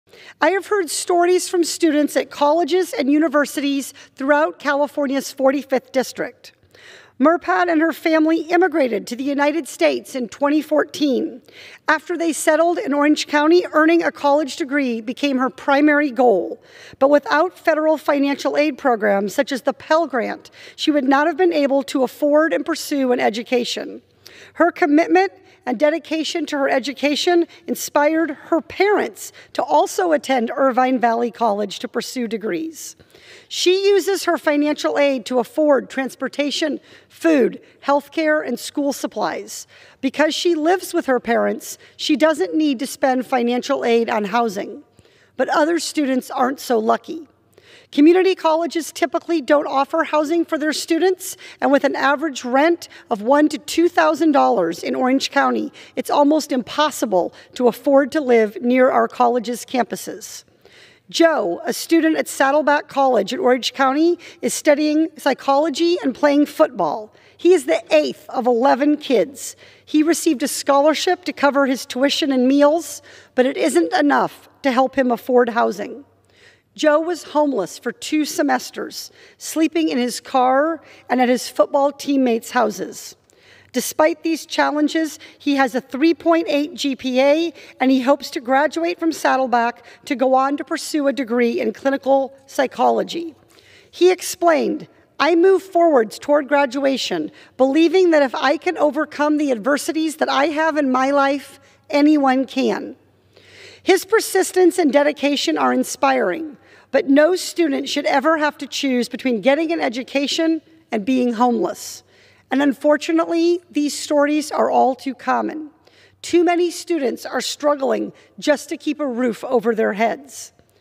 Rep. Porter highlights stories of OC community college students
Congresswoman Katie Porter told the stories of two students at Orange County community colleges during a speech on the House floor about college affordability.